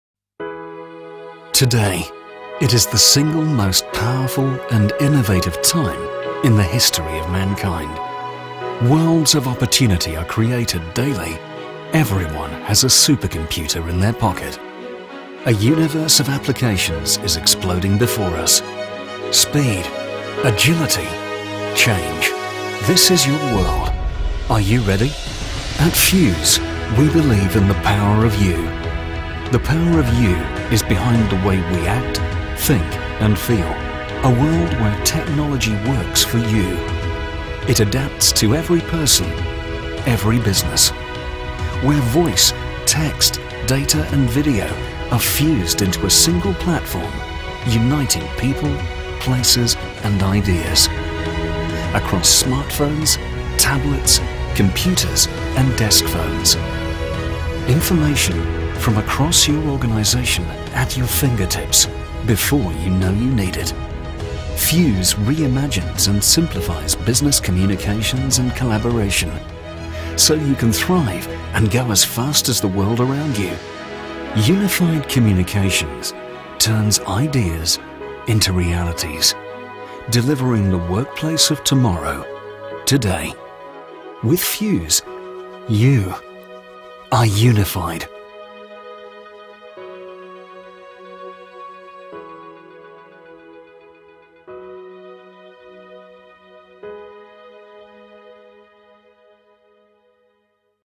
Fuze YOUnified Communications HD
The Fuze enterprise communications platform provides traditional PBX functionality of an on-premise telecom solution without the traditional burdens – capacity management, CAPEX spend, platform maintenance, and unpredictable costs from various access provide – to name a few.

We manage everything so you don’t have to.